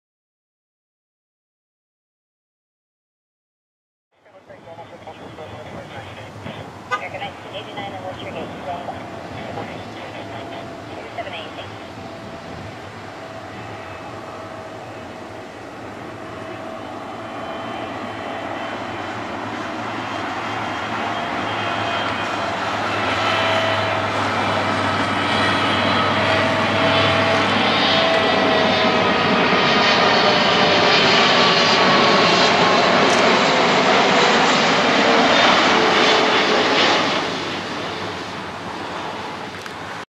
EVA Air 777-35E/ER [B-16711]
EVA Air 777-35E/ER landing on 6L at Los Angeles International Airport (LAX) on October 18, 2012